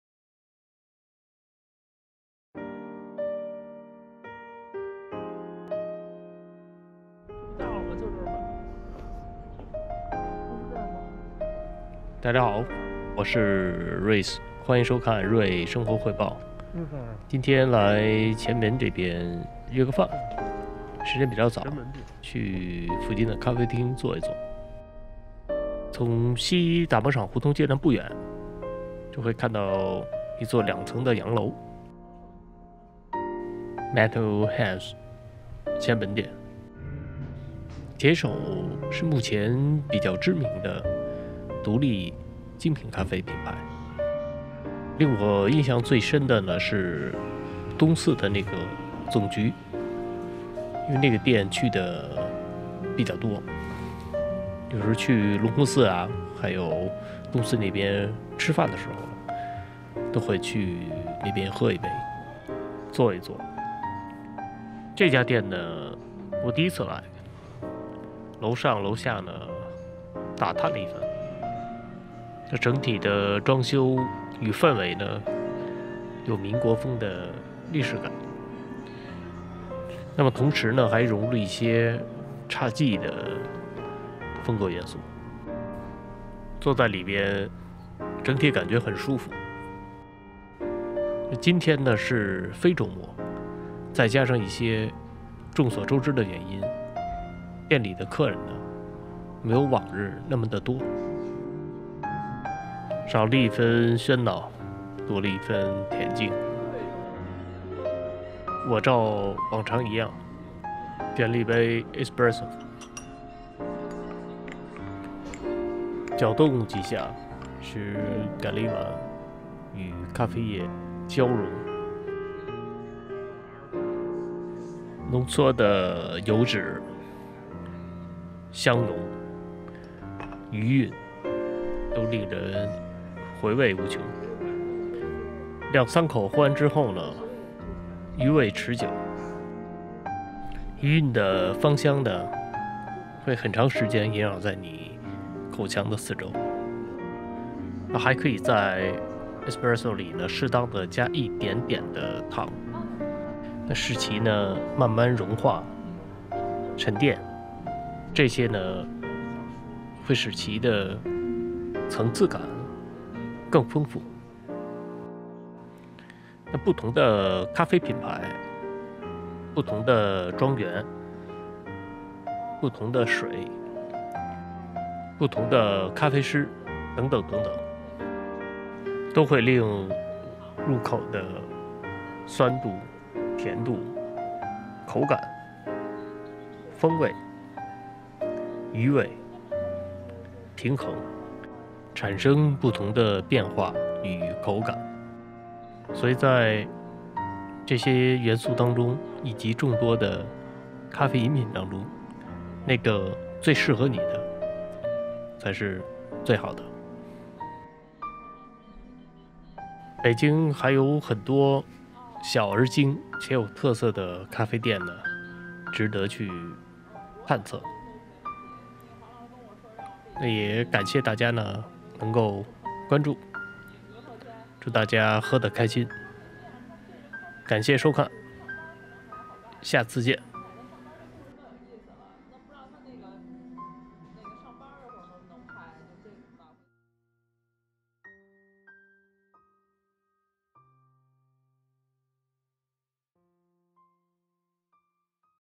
民国侘寂风咖啡馆 一杯espresso坐一坐 男人快乐如此简单
北京坊约饭
顺便在前门metal hands里坐一坐聊一聊喝一杯
男人快乐如此简单